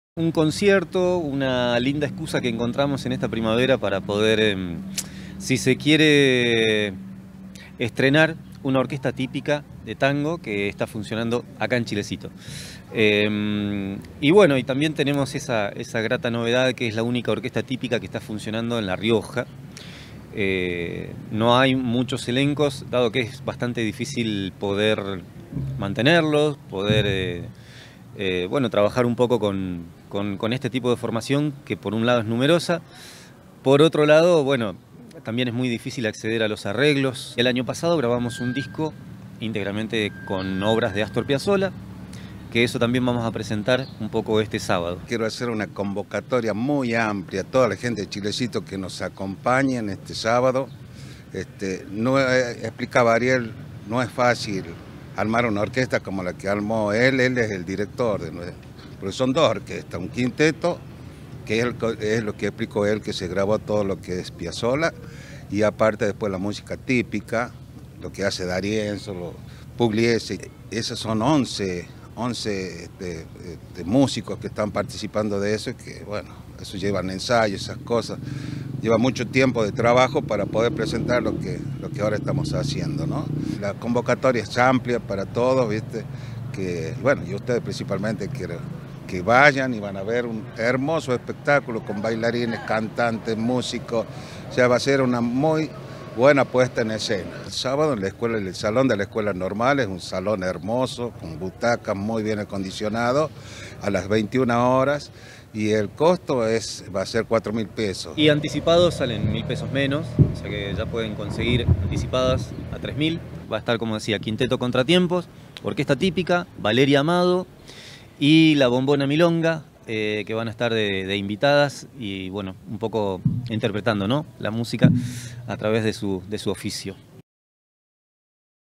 DiarioChilecito.com
Este sábado a las 21 hs, el salón de la Escuela Normal se viste de tango. Presentan quintetos, orquestas típicas, grupos de baile y artistas locales.
Consegúi tu entrada al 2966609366, o en Panadería 9 de Julio.